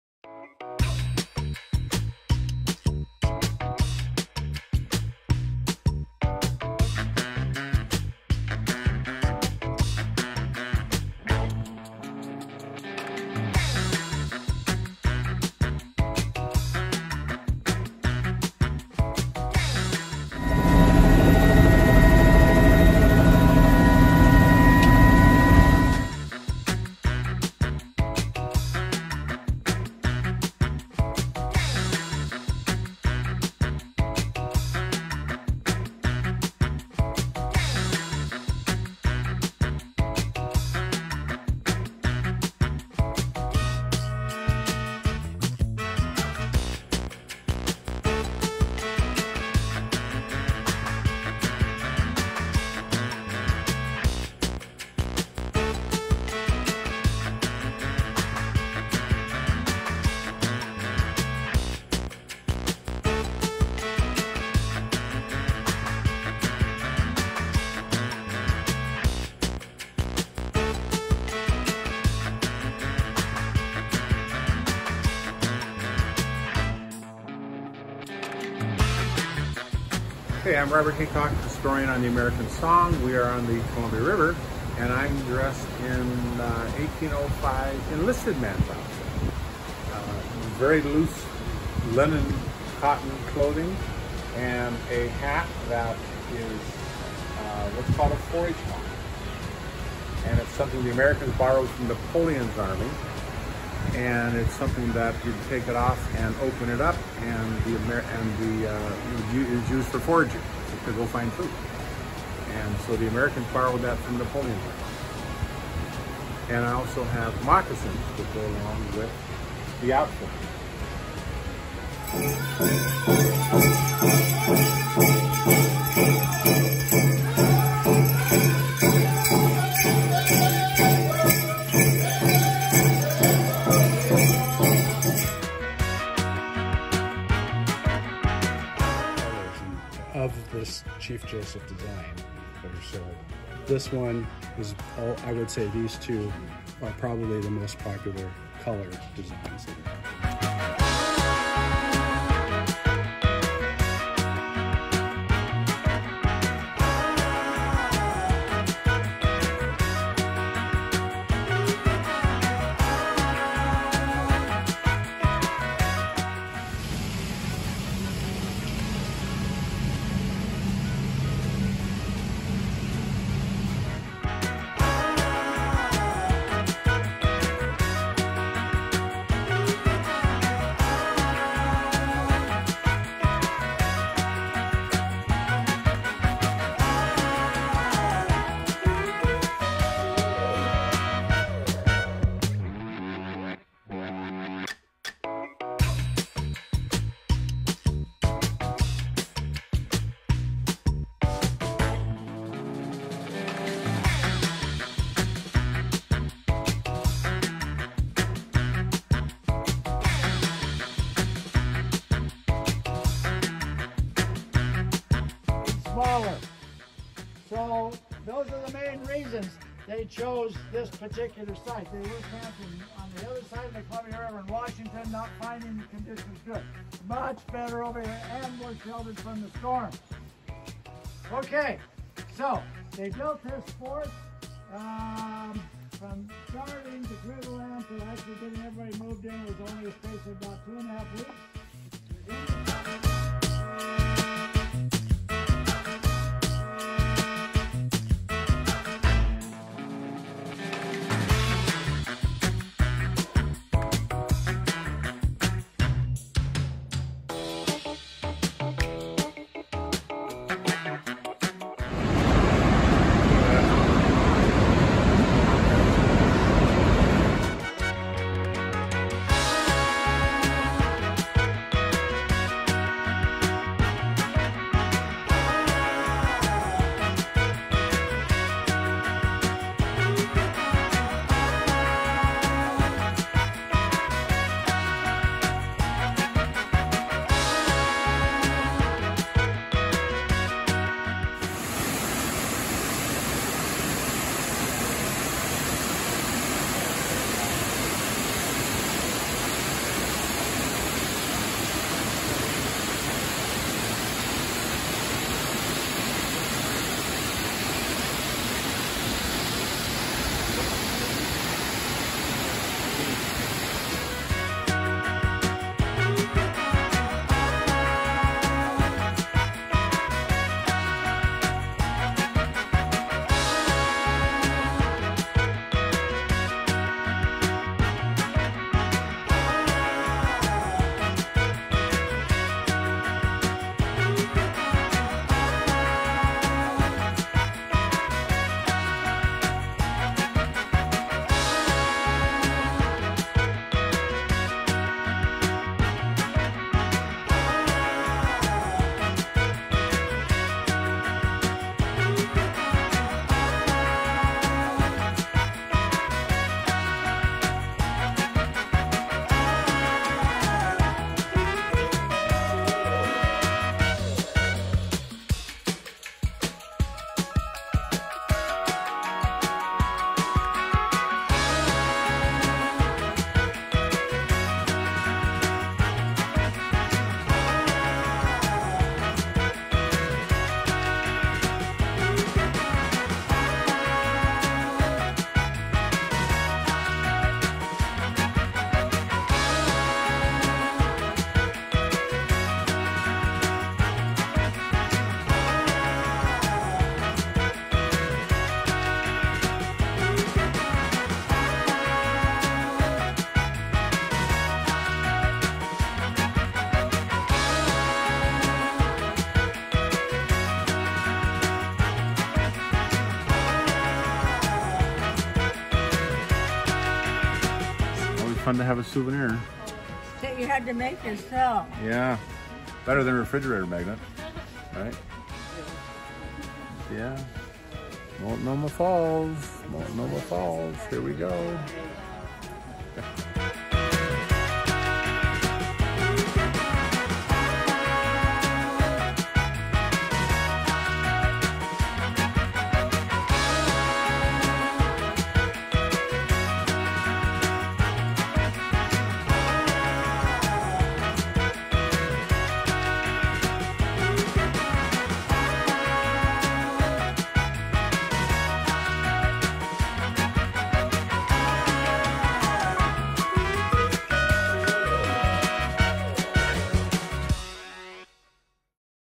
Columbia and Snake Rivers Cruise | American Cruise Lines
I was in awe of the spectacular ever-changing Pacific Northwest scenery that I saw from my private balcony as we cruised on the Snake and Columbia Rivers aboard the American Song. Watching the landscape transform from high dessert plains to lush coastal rainforests was a sight to behold that is permanently etched in my memory.

The 8-day Lewis & Clark cruise set sail from Clarkston, WA and went through 8 locks with a total of 567 miles traveled to Portland, OR.

Our River Historian’s daily presentations brought history to life as we followed in the footsteps of the legendary Lewis & Clark Expedition.

Everyday a new adventure unfolded in a different port of call. We participated in engaging guided shore excursions visiting Pendleton, Multnomah Falls, Mount St. Helens, Bonneville Dam, The Dalles, Fort Clatsop, Astoria and more.

There was never a dull moment on land or water - from enrichment activities including watercolor classes, cooking demonstrations and historic lectures, to live entertainment and nightly cocktail parties.